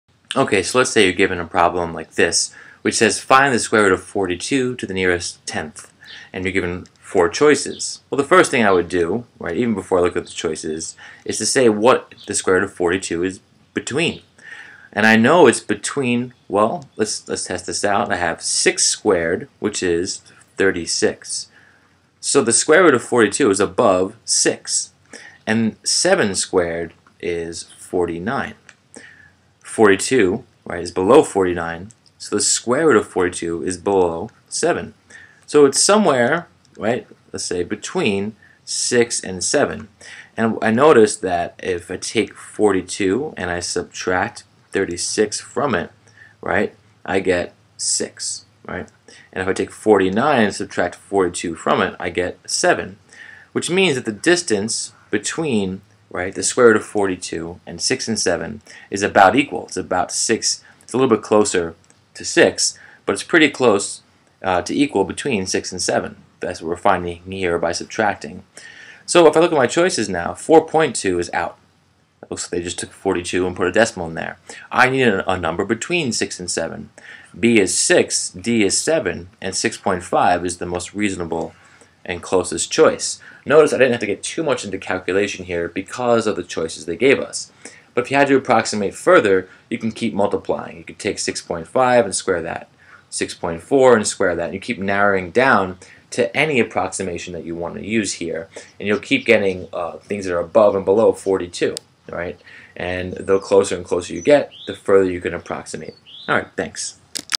17 Square Root of 42 #32
Here we look at some typical square root examples